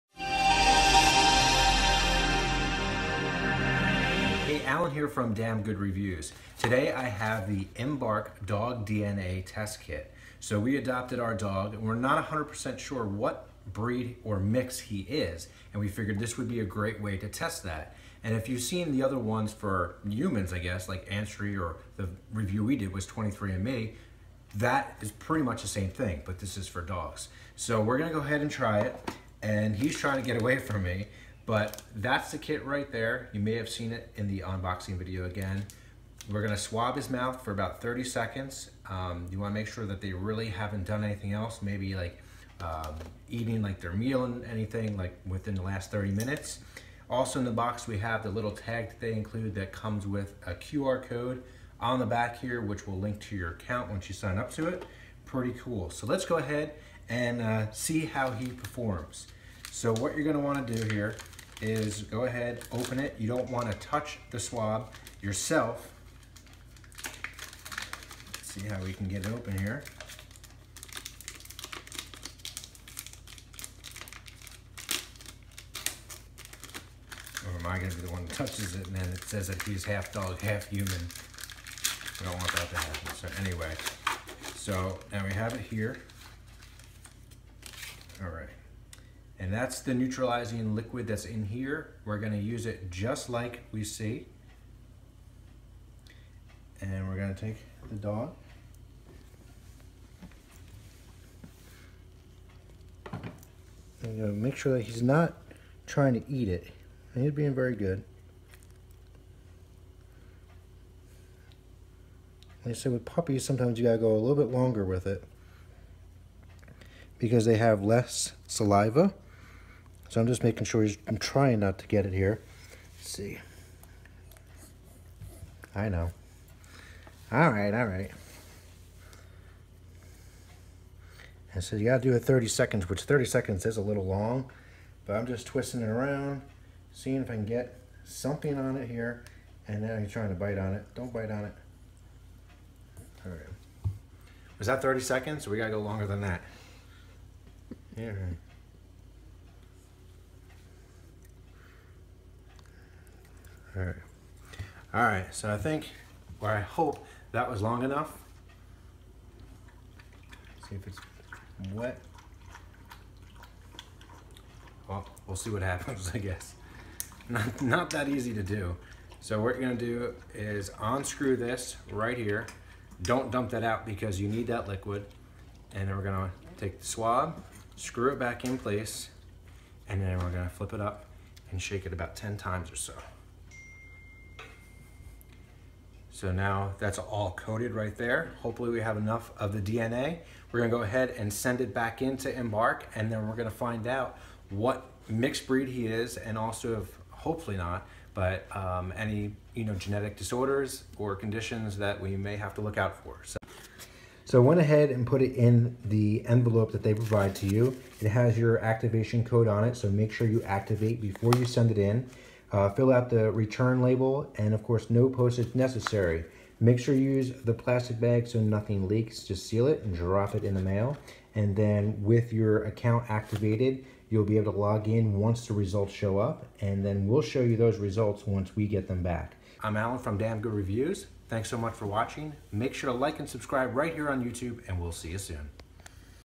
Embark Dog DNA Test Kit Demo - Dog DNA Sample Collection
We recently adopted a new dog and the shelter was not 100% sure what his mix was. Well we wanted to know and we are putting the Embark Dog DNA Test Kit, well, to the test.

In this video we will perform the DNA collection using the EMBARK DNA Test Kit.